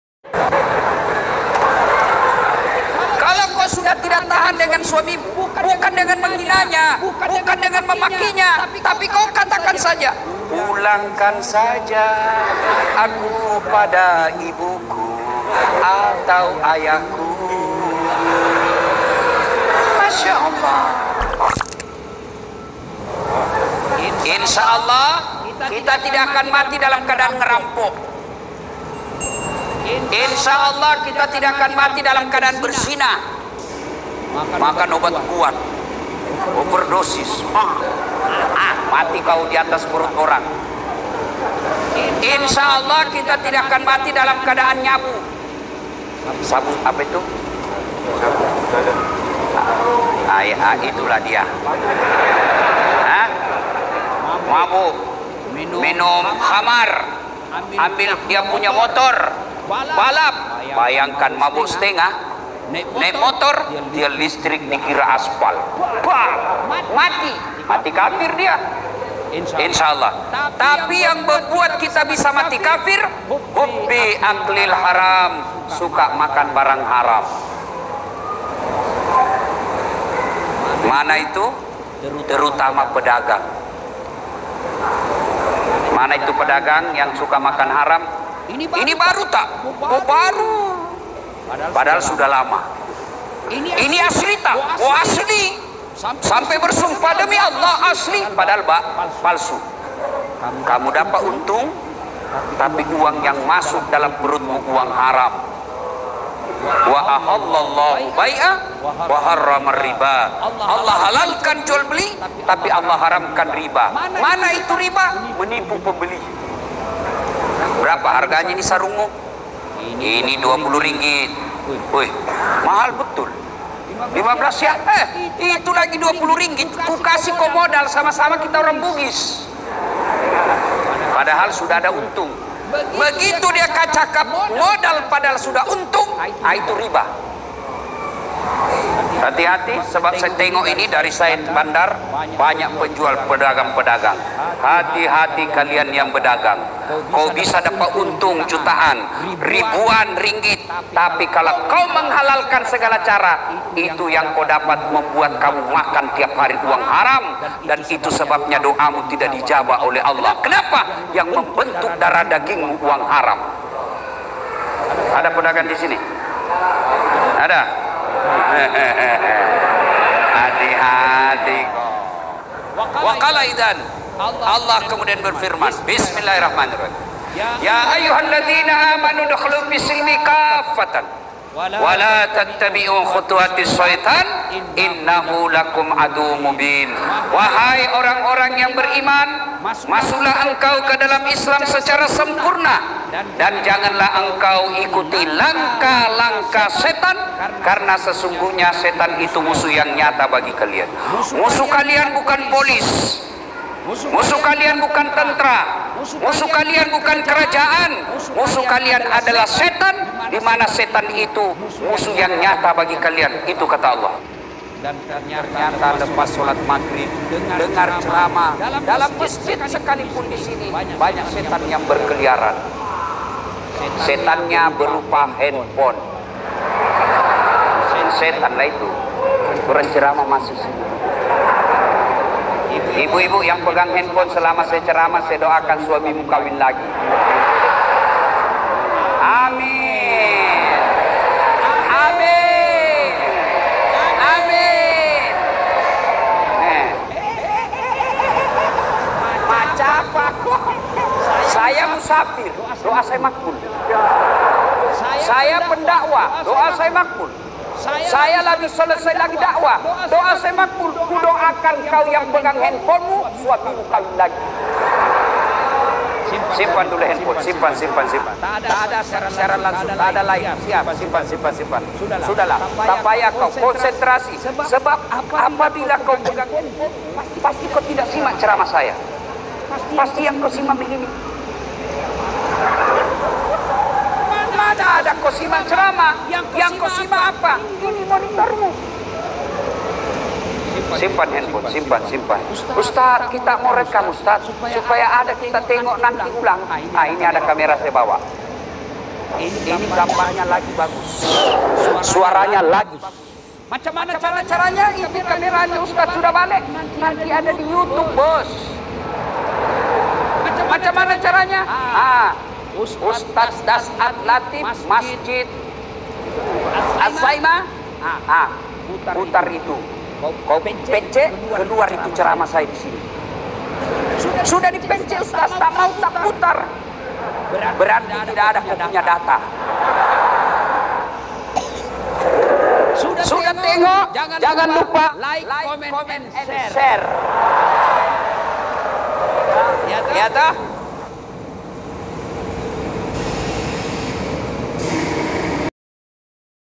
kalau kau tidak suka dengan suamimu pulangkan saja ke orang tuanya
Assalamualaikum dan salam sejahtera kepada sahabat semua

Pada video kali ini adalah tentang ceramah ustadz Das'ad latif